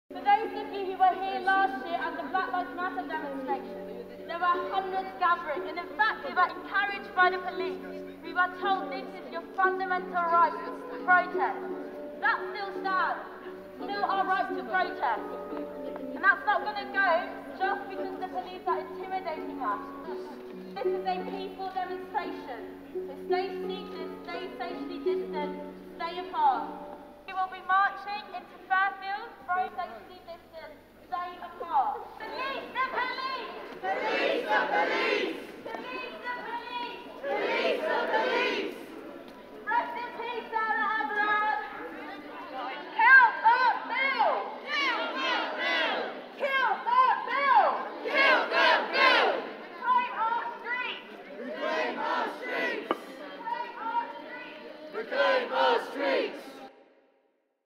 Kill The Bill - Kingston upon Thames
A peaceful, masked, socially distanced KillTheBill protest in Kingston upon Thames yesterday. @BLMKingston 💛